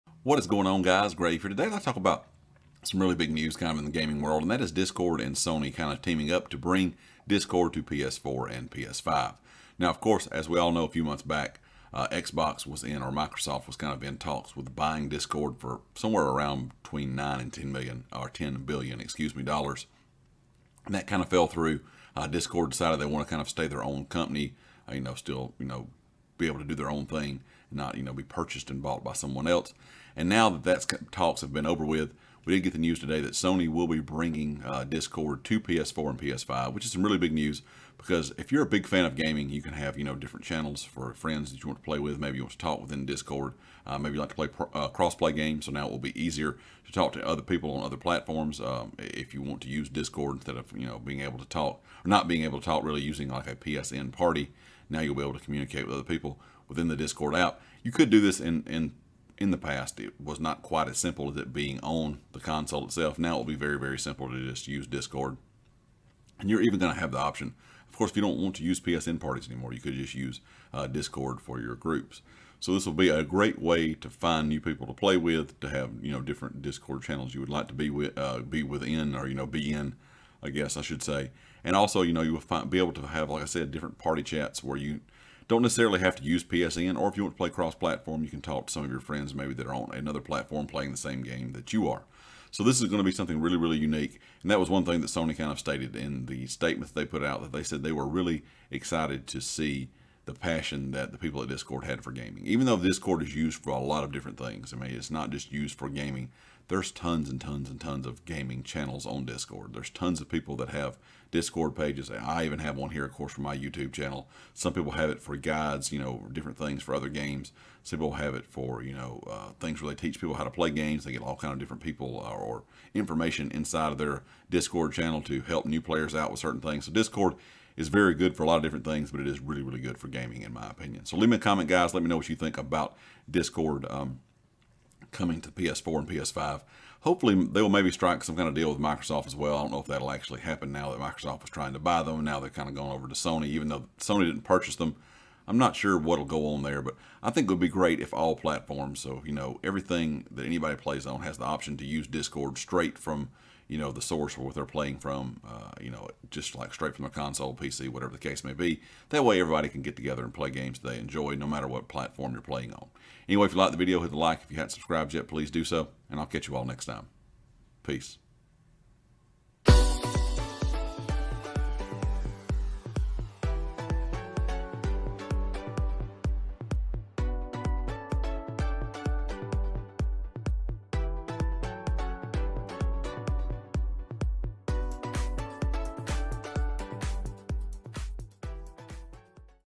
Discord Coming To PS4 & PS5
Discord will be added to ps4 and ps5 early next year. This will be great for cross play.